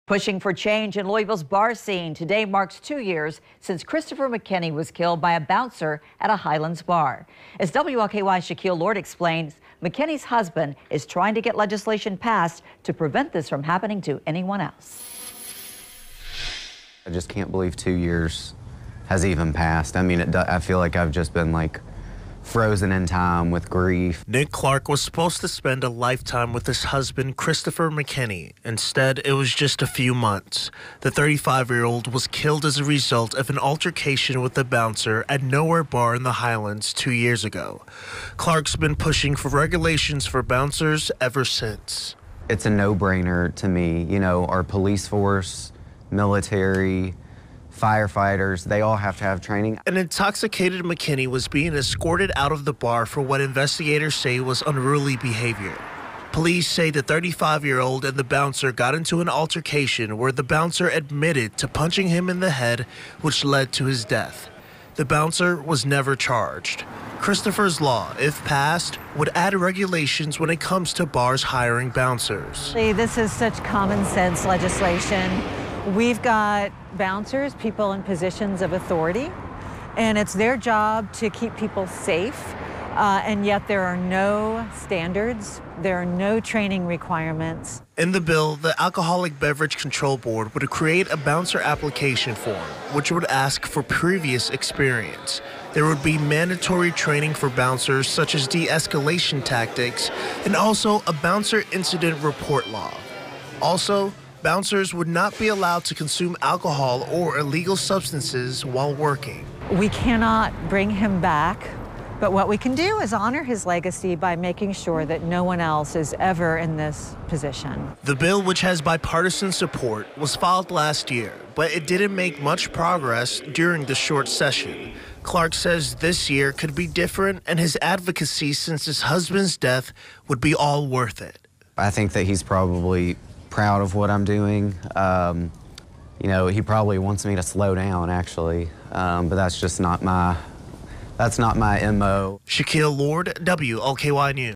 After death of Louisville man at Highlands bar, proposed bill would require training for bouncers
The 35-year-old McKinney was killed as a result of an altercation with a bouncer at Nowhere Bar in the Highlands two years ago. Clark's been pushing for regulations for bouncers ever since.